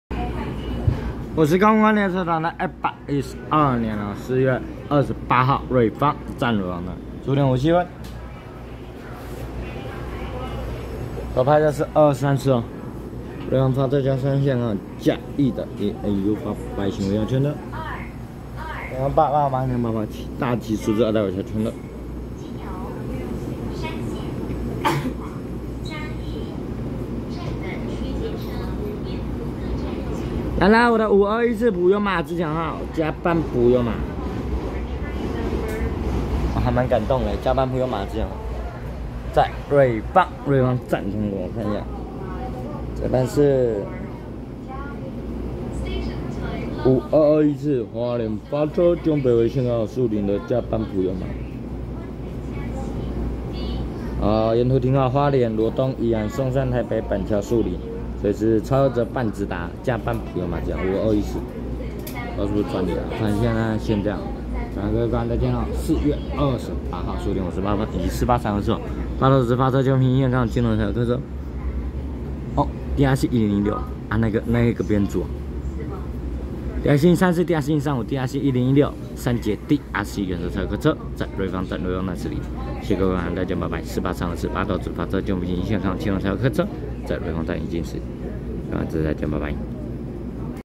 5221次花蓮發車經由北迴線開往樹林的加班TEMU2033+TEMU2034車王普悠瑪自強號，2243次瑞芳發車經由山線開往嘉義的EMU887+EMU888大吉數字二代微笑號區間車
112/4/28瑞芳站15:57分拍攝5221次花蓮發車經由北迴線開往樹林的加班TEMU2033+TEMU2034車王普悠瑪自強號，2243次瑞芳發車經由山線開往嘉義的EMU887+EMU888大吉數字二代微笑號區間車